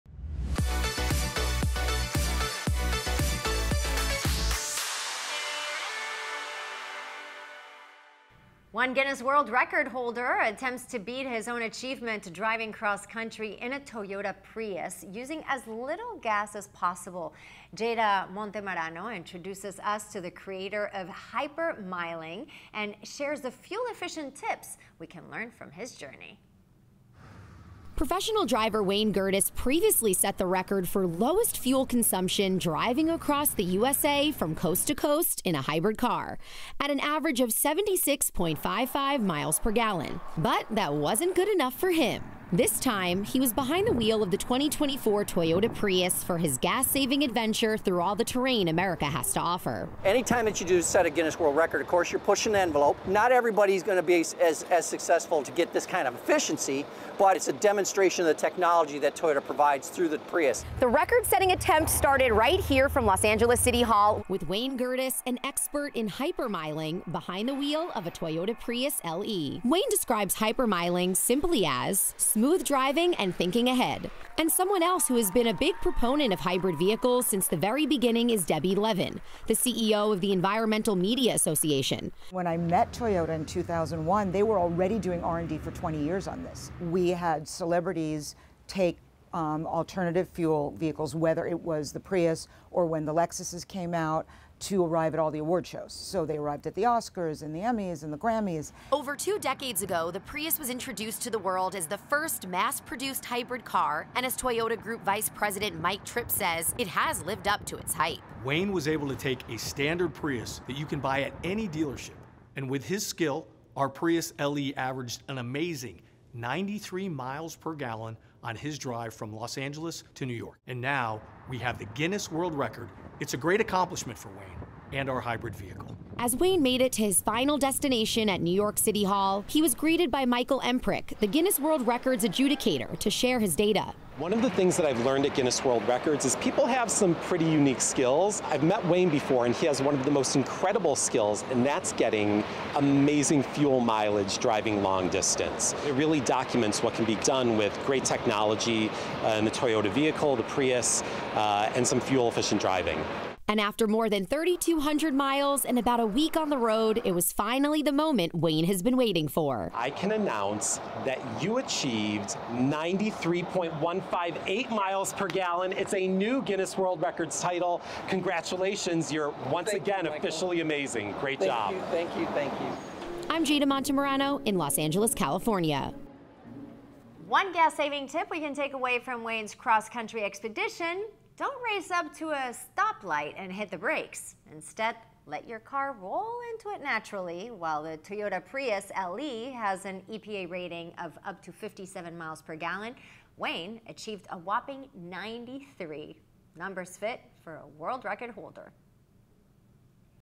Toyota World Record
Learn about one man’s attempt to set a world record by driving cross-country in a Toyota Prius using as little gas as possible.